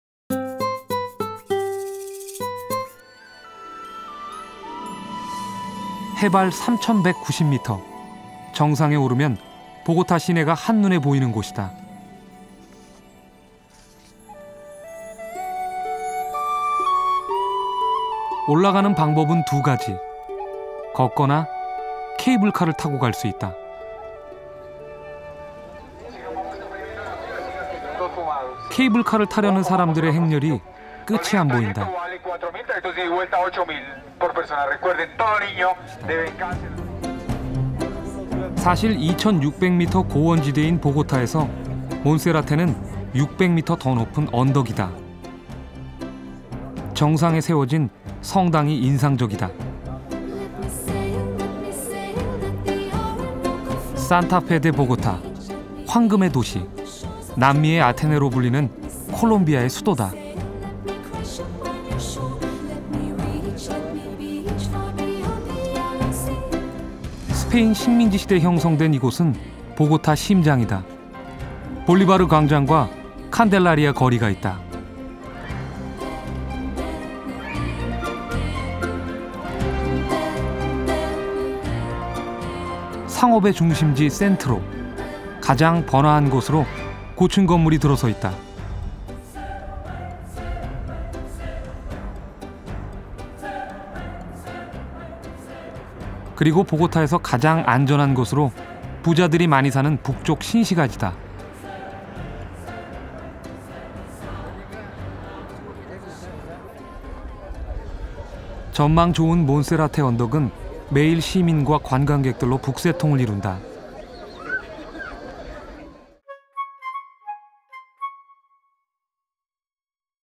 【K】Colombia Travel-Bogota[콜롬비아 여행-보고타]몬세라테 언덕/Monserrate Hill/Observatory/Cable car/Lift
[한국어 정보]
해발 3190m, 정상에 오르면 보고타 시내가 한눈에 보이는 곳이다. 올라가는 방법은 두 가지, 걷거나 케이블카를 타고 갈 수 있다. 케이블카를 타려는 사람들의 행렬이 끝이 안 보인다. 사실 2600m 고원지대인 보고타에서 몬세라테는 600m 더 높은 언덕이다. 정상에 세워진 성당이 인상적이다. 산타페 데 보고타! 황금의 도시! 남미의 아테네로 불리는 콜롬비아의 수도다. 스페인 식민지시대 형성된 이곳은 보고타 심장이다. 볼리바르 광장과 칸델라리아 거리가 있다. 상업의 중심지 센트로! 가장 번화한 곳으로 고층 건물이 들어서 있다. 그리고 보고타에서 가장 안전해 부자들이 많이 사는 북쪽 신시가지다. 전망 좋은 몬세라테 언덕은 매일 시민과 관광객들로 북새통을 이룬다.

Elevation 3190m, is where the city is visible at a glance Bogota rises to the top. There are two dating methods, you can walk or ride the cable car. Taryeo the cable car seems not the end of the procession of people. In fact Monserrate in Bogota 2600m plateau of 600m is higher hills. The impressive cathedral is built on top. Santa Fe de Bogota! City of Gold! The capital of Colombia, known as the Athens of South America. This is a Spanish colonial era formed the heart Bogota. There is Bolivar Square and Candelaria street. Centro's commercial center! The high-rise buildings are heard in the most bustling place. And the most safe a lot of wealthy people living in the north of Bogota Dessau. Monserrate Hill with views form the daily crowded with citizens and tourists.

3190m de elevación, es donde la ciudad es visible a simple vista Bogotá sube a la cima. Hay dos métodos de datación, se puede caminar o viajar en el teleférico. Taryeo el teleférico no parece el final de la procesión de la gente. De hecho Monserrate en Bogotá 2600m meseta de 600m es colinas más altas. La impresionante catedral se construyó en la parte superior. Santa Fe de Bogotá! Ciudad de Oro! La capital de Colombia, conocida como la Atenas de América del Sur. Esta es una época de la colonia española formada el corazón de Bogotá. Existe la Plaza Bolívar y la calle Candelaria. El centro comercial de Centro! Los edificios altos se escuchan en la más bulliciosa lugar. Y lo más seguro un montón de gente rica que vive en el norte de Bogotá Dessau. Monserrate colina con vistas a formar el concurrido diariamente con los ciudadanos y los turistas.

[Information]
■클립명: 남미012-콜롬비아01-01 몬세라테 언덕/Monserrate Hill/Observatory/Cable car/Lift
■여행, 촬영, 편집, 원고: 채형석 PD (travel, filming, editing, writing: KBS TV Producer)
■촬영일자: 2008년 1월 January

[Keywords]
,남미,America,아메리카,콜롬비아,Colombia,Colombia,,채형석,2008,1월 January,보고타,Bogota,Bogota,